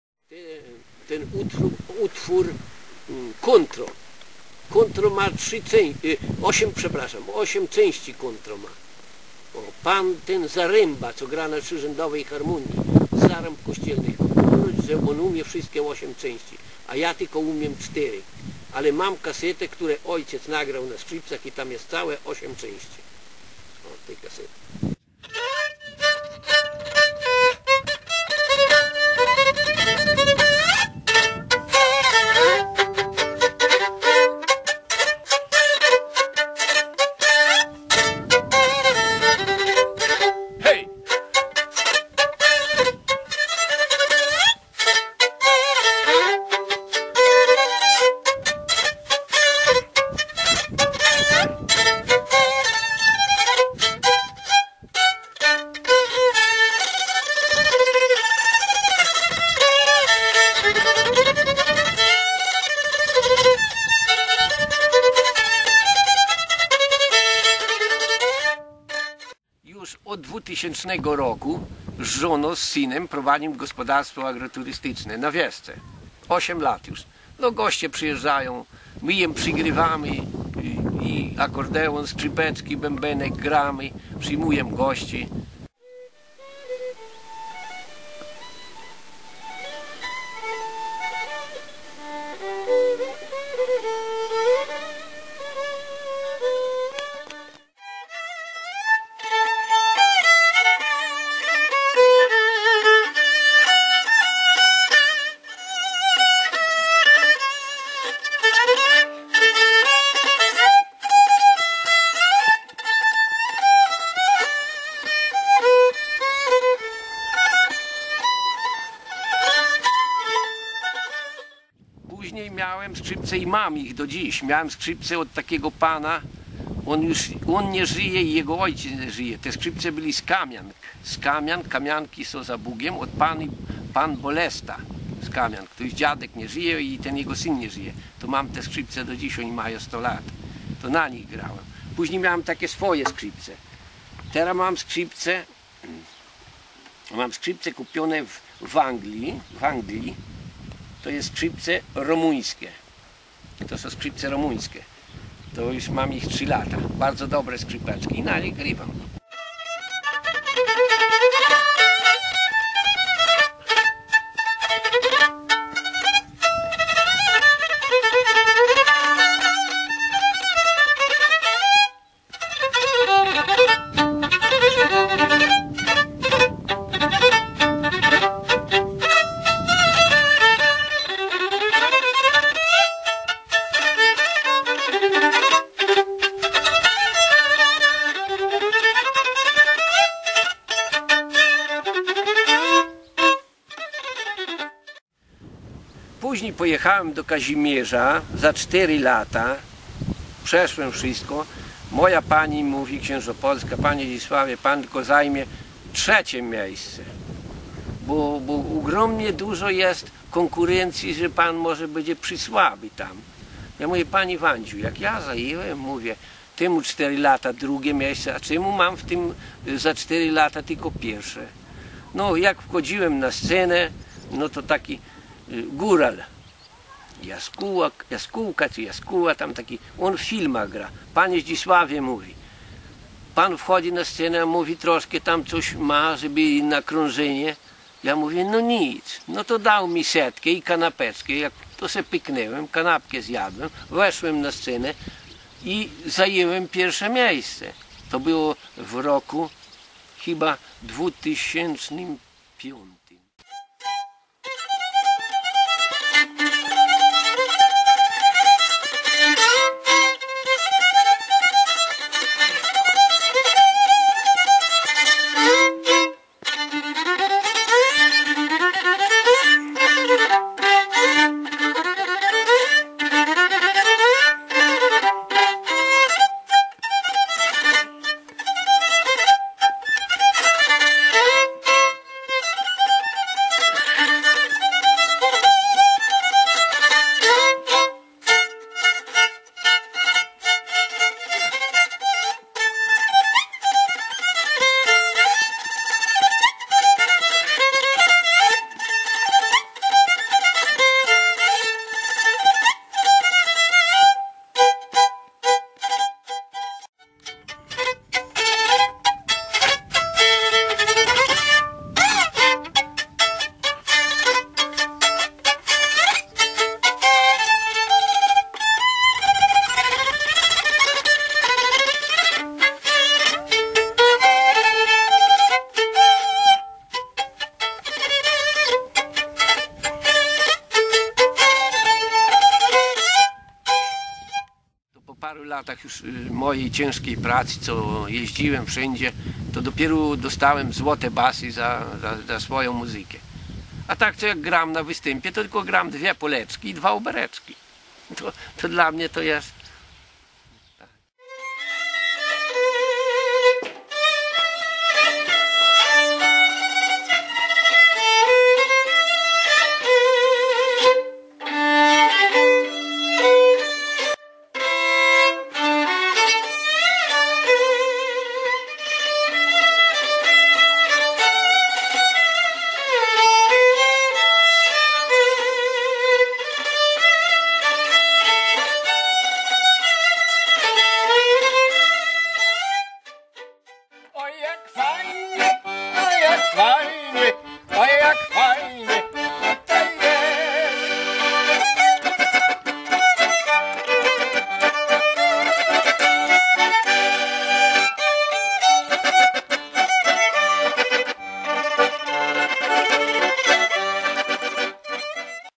Siemiatycze - "Muzykanci - Zdzisław Radzikowski" cz. 2
Traditional music from Poland - Podlachia
Pierwsza część krótkiego filmu z cyklu "Muzykanci", ktorego bohaterem jest Zdzisław Radzikowski, skrzypek pochodzacy ze wsi Granne w powiecie siemiatyckim, a mieszkający we wsi Wieska w powiecie sokołowskim.
Wraz z żona prowadzi też tam gospodarstwo agroturystyczne.

zdjęcia Ewa Magdalena Iwaniak
montaz i realizacja Jerzy Nowicki